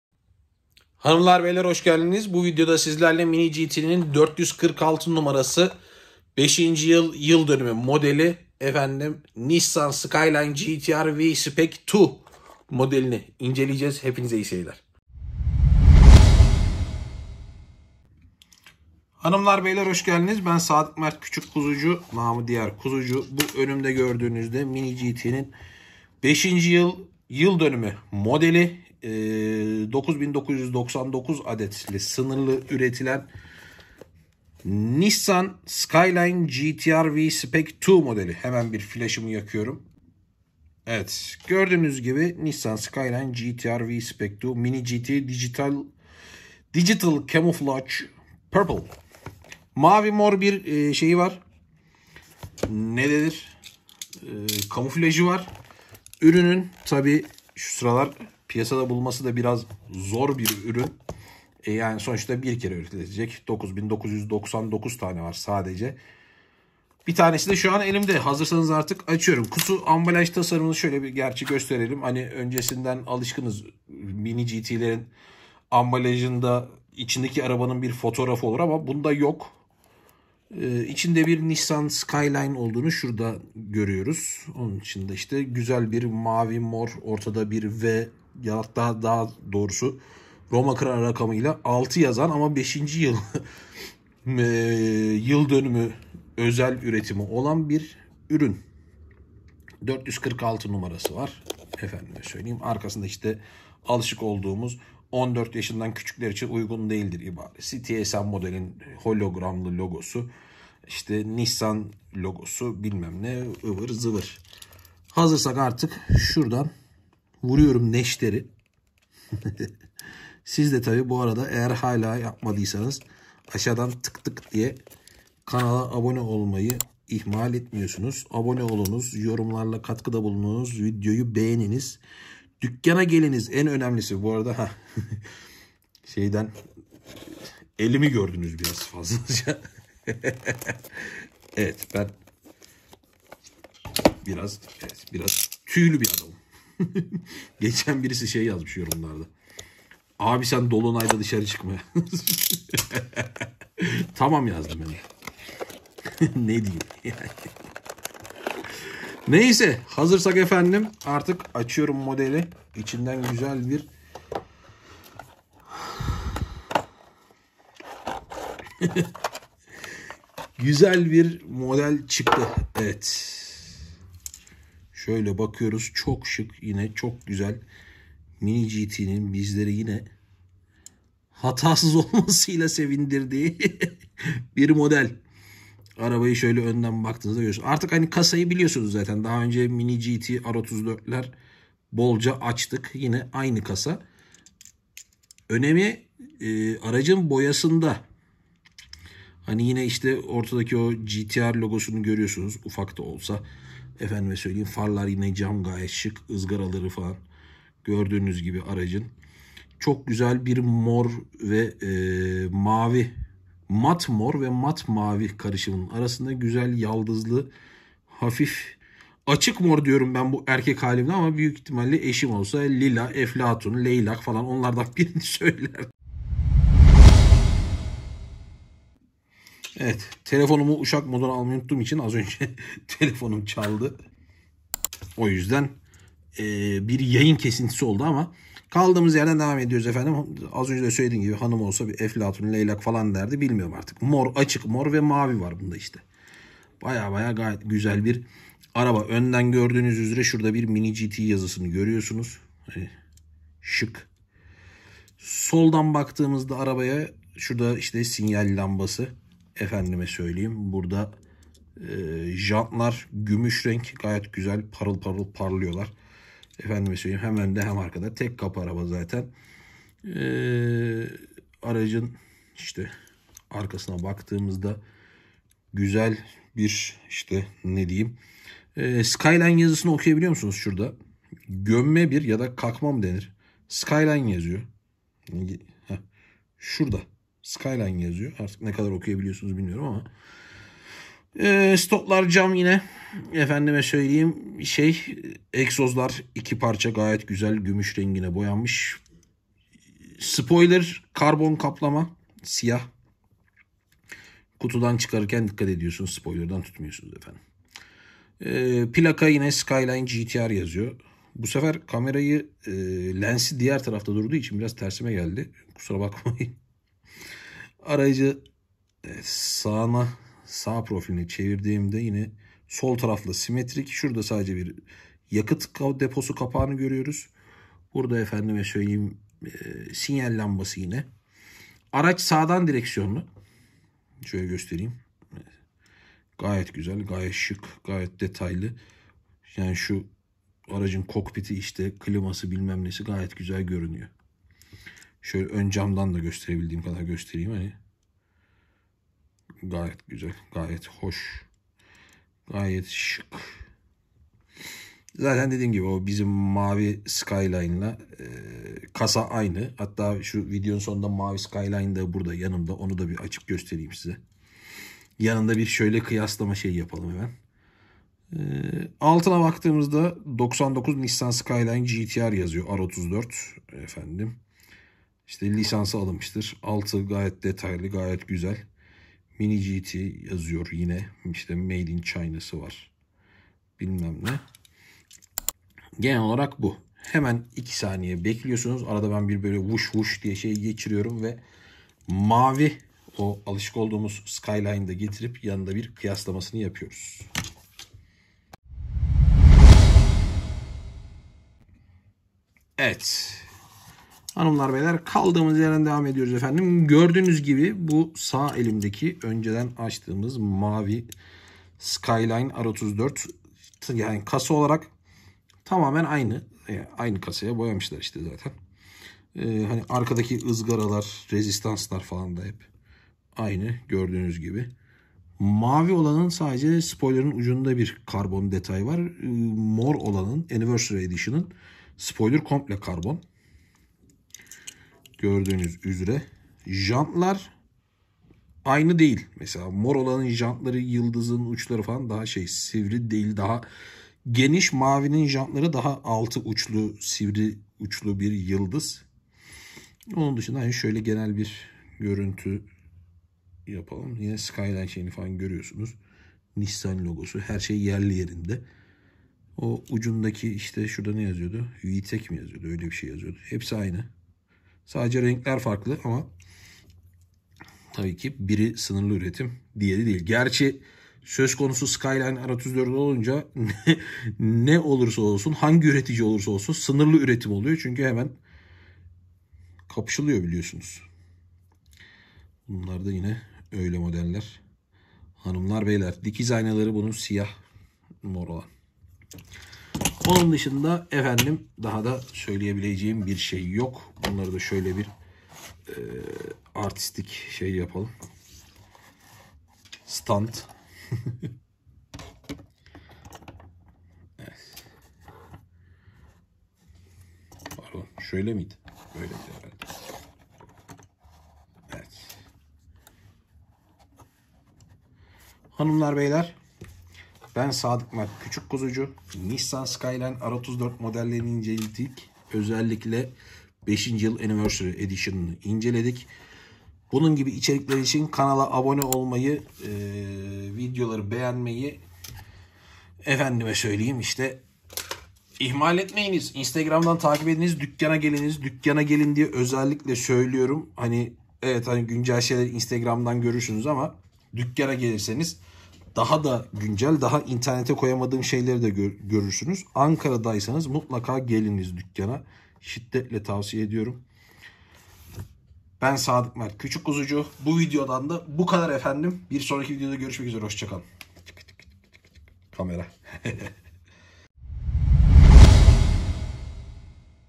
Mini GT Nissan Skyline GT-R (R34) V-Spec II MINI GT Digital Camouflage Purple İNCELEME
Nissan Skyline GT-R (R34) V-Spec II MINI GT Digital Camouflage Purple 5. Yıl Dönümü modeli ile karşınızdayım.

Ürünü nerede bulacağınızı biliyorsunuz ^_^

Dükkana gelin, dükkana.